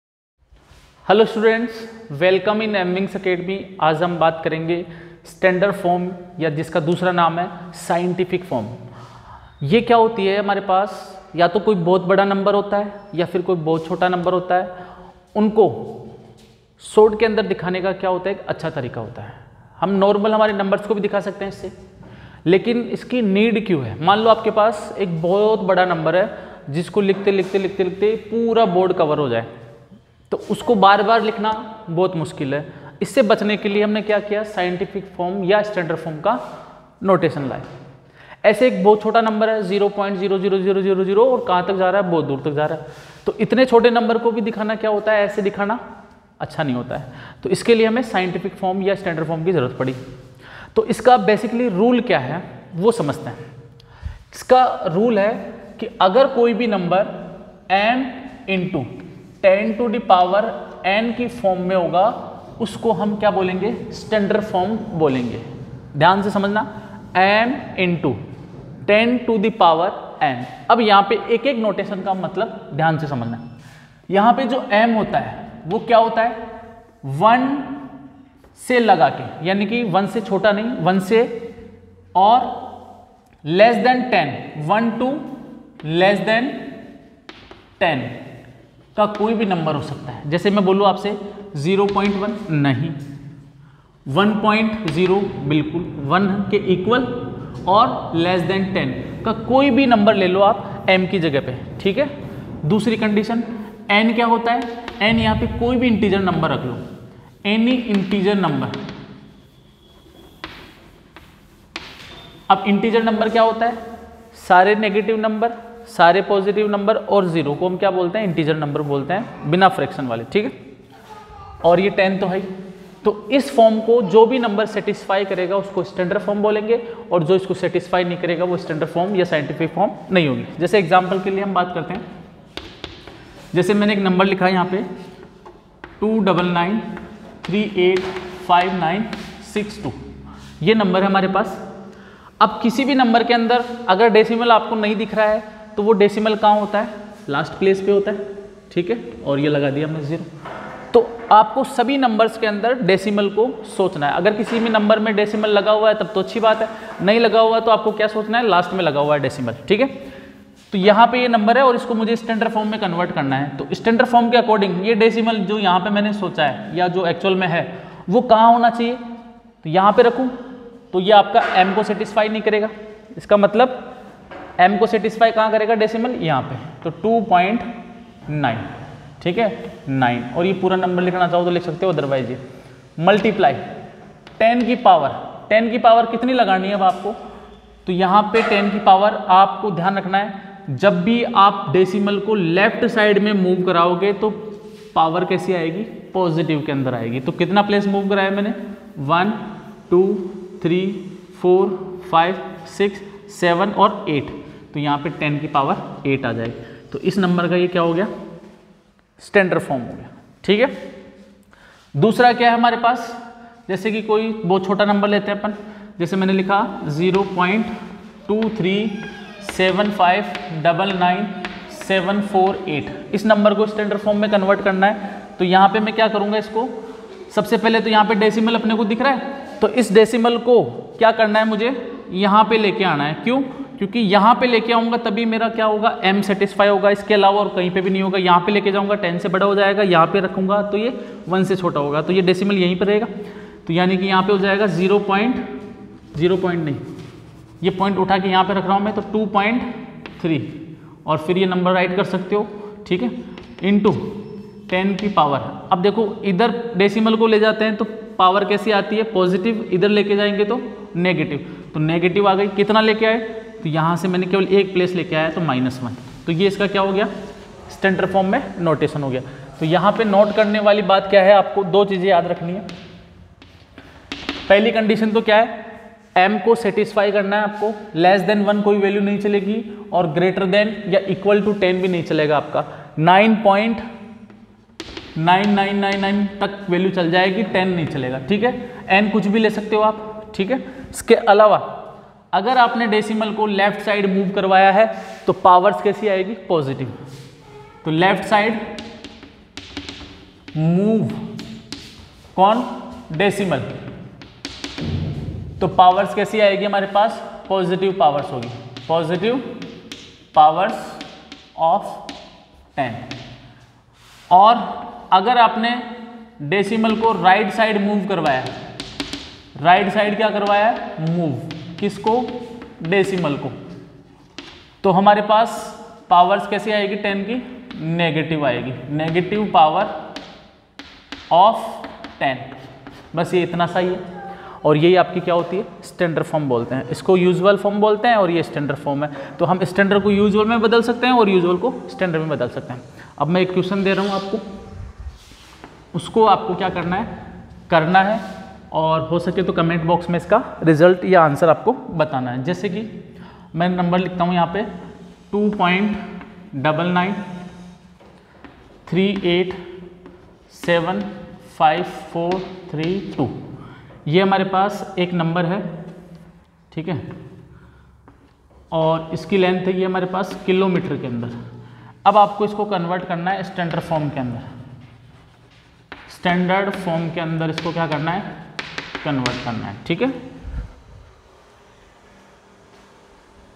Standard form kya hai || Scitific form in easy way || super trick
What is Standard form?
                  or
what is scitific form?
☆Standard form is a way of writing down very large or very small numbers easily. 103 = 1000, so 4 × 103 = 4000 . So 4000 can be written as 4 × 10³ . This idea can be used to write even larger numbers down easily in standard form. Small numbers can also be written in standard form.

 Thanks:
M. Wings Academy
by mahi sir